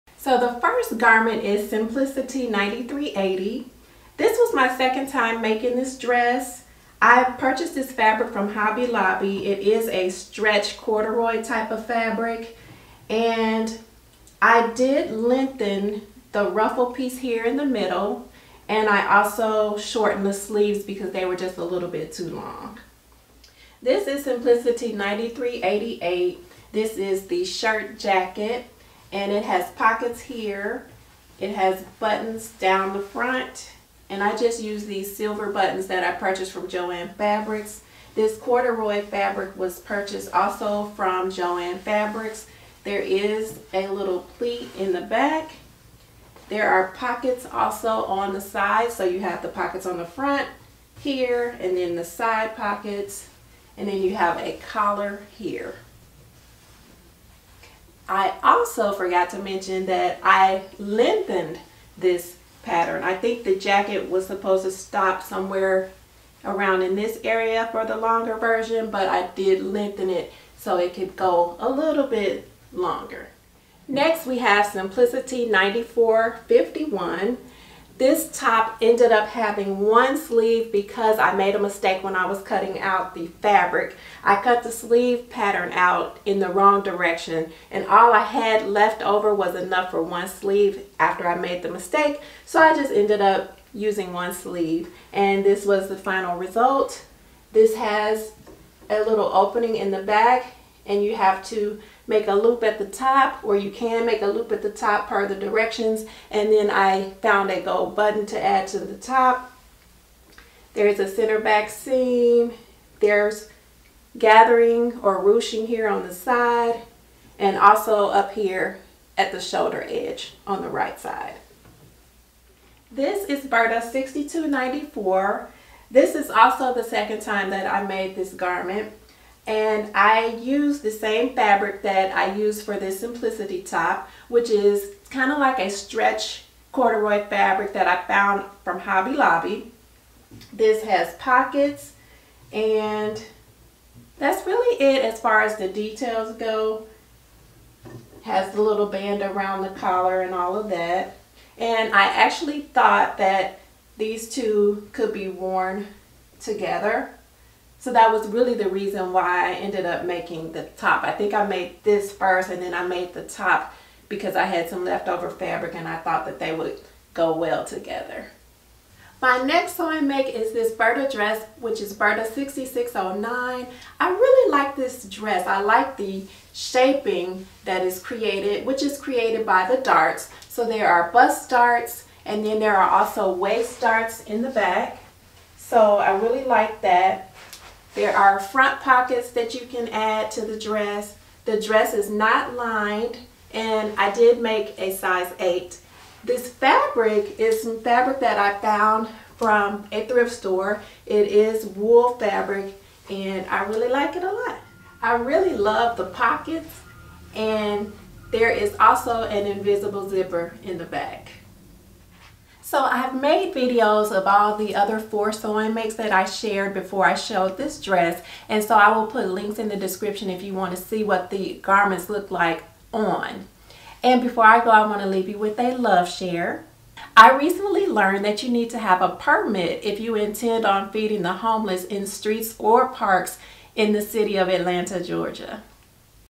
MARCH SEWING MAKES & A dress you haven’t seen, BURDA 6609
I have March sewing makes to share plus a new sewing make, Burda 6609.  For the month of March I shared Simplicity 9380, Simplicity 9451, Burda 6294, and Simplicity 9388.  I gathered all of these makes and talked a little bit about each one.  Below are videos of each garment sharing what they each look like on.